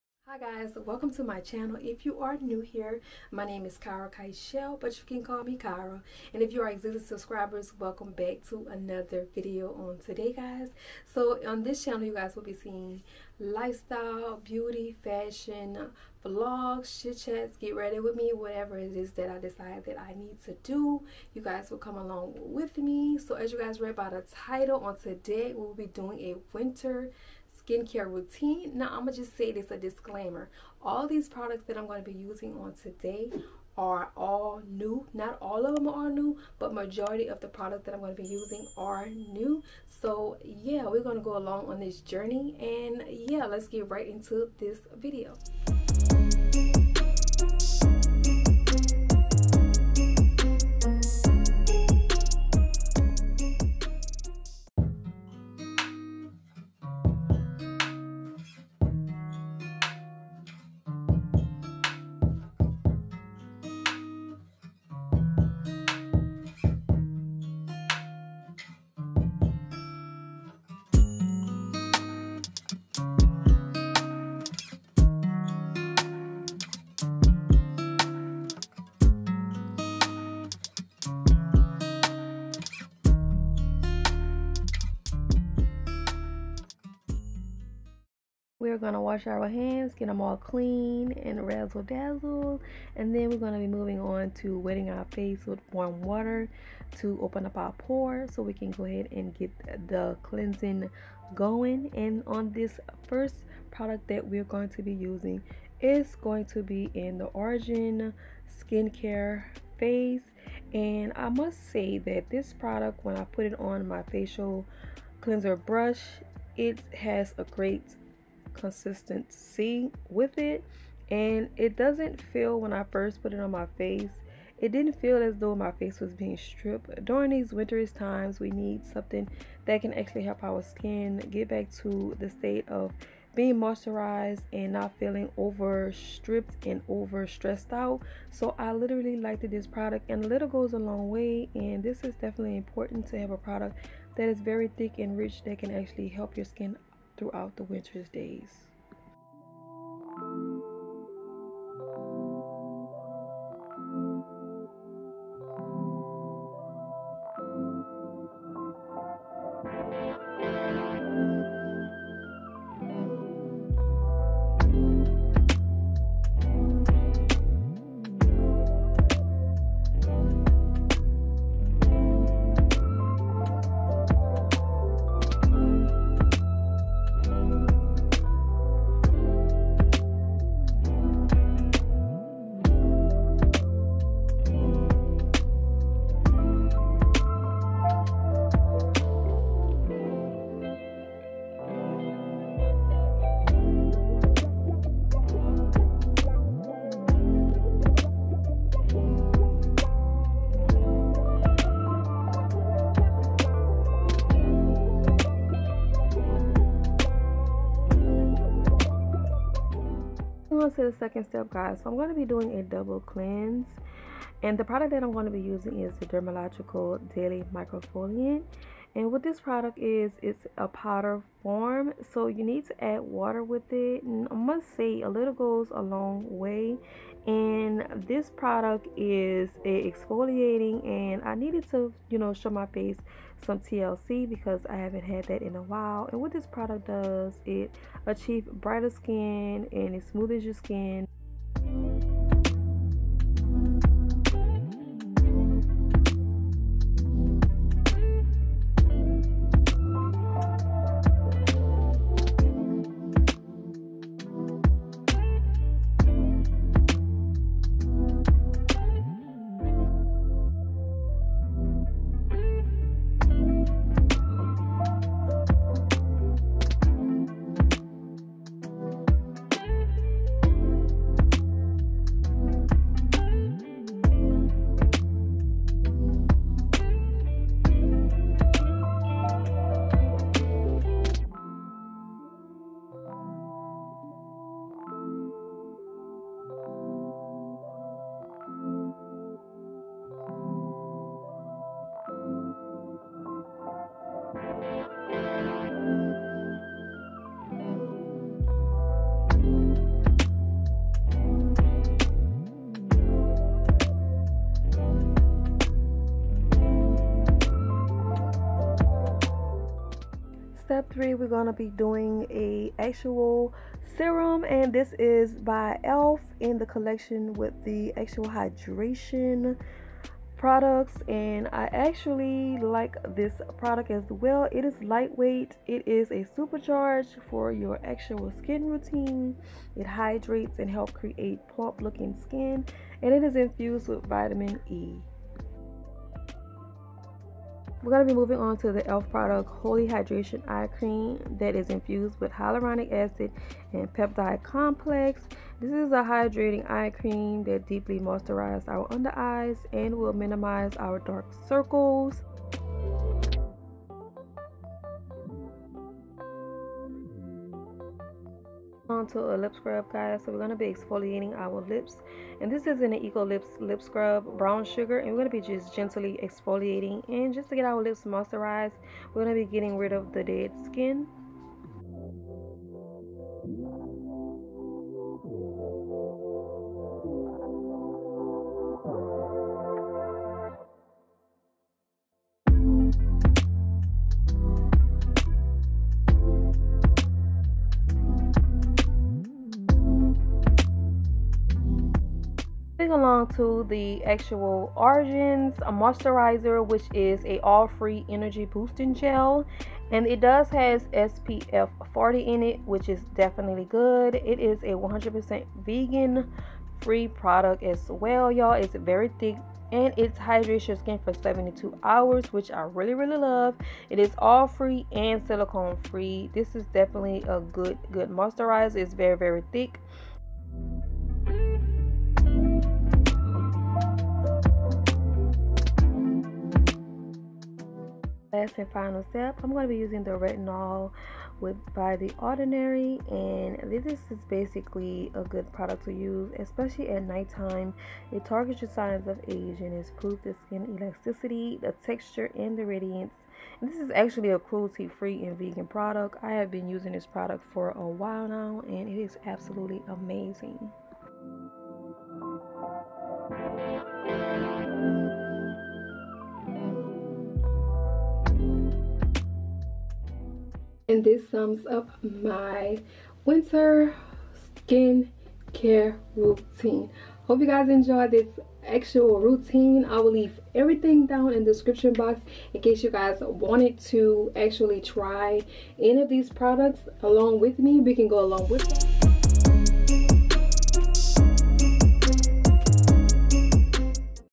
VLOGMAS DAY 7~Winter Skin Care Routine | karakichelle
✨Hey Y'all, It's Kara!✨
In today's video I will be showing you guys my winter skin care routine +new products I will be using.

Products Used:

▪️ORIGIN+Checks&Balance
▪️Dermalogica Daily Microfoliant
▪️E.L.F Booster Serum
▪️E.L.F Eye Cream
▪️ORIGIN GinZing Oil+Free Energy+Boosting   Gel Moisturizer
▪️Eco Lips+Lip Scrub
▪️The Ordinary+Retinol
▪️Carmex